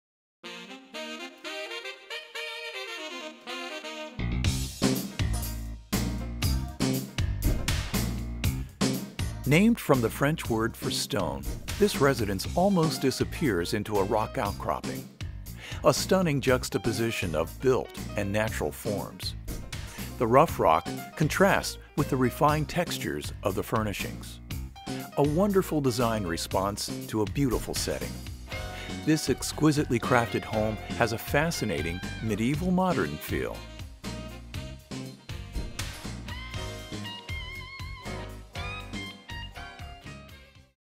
The Pierre | Olson Kundig Architects - Institute Honor Awards for Architecture
2014 Recipient | Institute Honor Awards for Architecture - The Pierre | Olson Kundig Architects
The owner’s affection for a stone outcropping on her property and the views from its peak inspired the design of this house. Conceived as a bunker nestled in the rock, the Pierre (French for stone) celebrates the materiality of the site. From certain angles, the house—with its rough materials, encompassing stone, green roof, and surrounding foliage—almost completely disappears into nature.